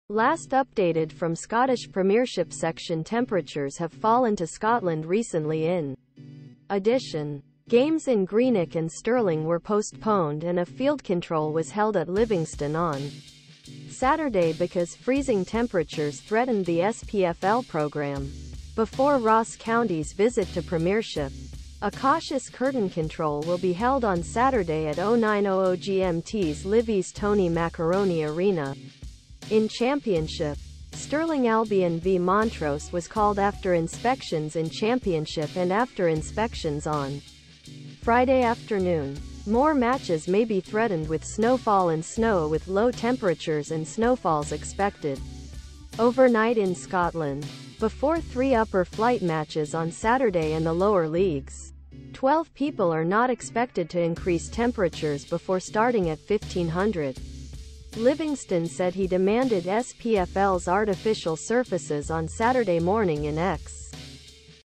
SPFL: Matches in Ayr, Greenock and Stirling off with pitch inspection for Livingston v Ross County
Last Updated from Scottish Premiership section  Temperatures have fallen to Scotland recently In addition, games in Greenock and Stirling were postponed and a field control was held at Livingston on Saturday because freezing temperatures threatened the SPFL program. Before Ross County's visit to Premiership, a "cautious curtain control" will be held on Saturday at 0900 GMT's Livi's Tony Macaroni Arena. In championship, Stirling Albion V Montrose was called after inspections in championship and after inspections on Friday afternoon. More matches may be threatened with snowfall and snow with low temperatures and snowfalls expected overnight in Scotland. Before three upper flight matches on Saturday and the lower leagues, 12 people are not expected to increase temperatures before starting at 1500. Livingston said he demanded SPFL's artificial surfaces on Saturday morning in X.